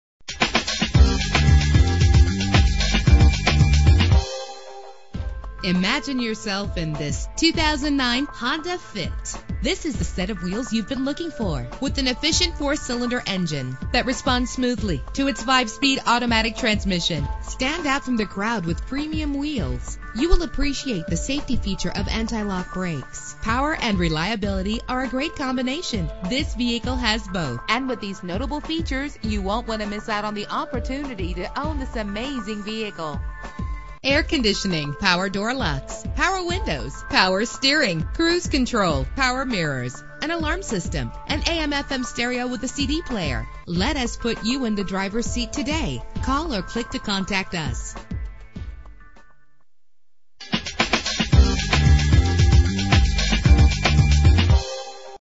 2009 Honda Fit - Bellevue WA
Year: 2009
Make: Honda
Model: Fit
Trim: Sport
Engine: 1.5 liter inline 4 cylinder 16 valve
Transmission: 5-Speed Automatic
Color: Copper
Mileage: 23468
Address:
15000 SE Eastgate Way